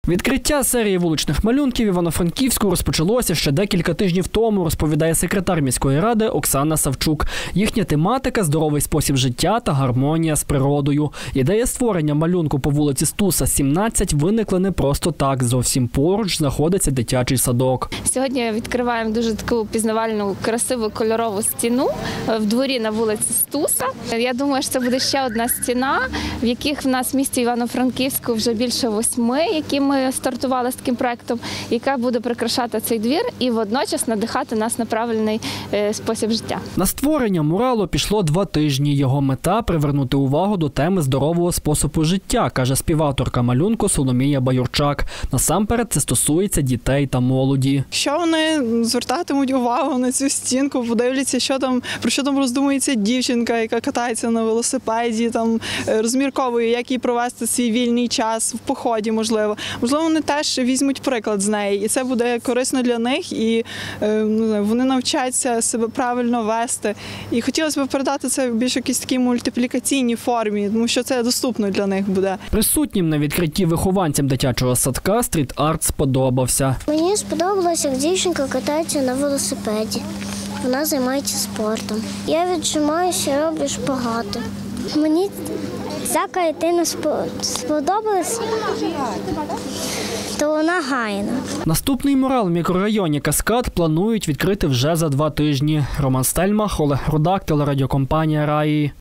В Івано-Франківську створили новий мурал
В Івано-Франківську презентували новий стріт-арт. Малюнок з'явився на фасаді будівлі по вулиці Стуса.
Художники кажуть - створили його для дітей, адже неподалік є дитячий садочок.

Бачили мурал і наші журналісти.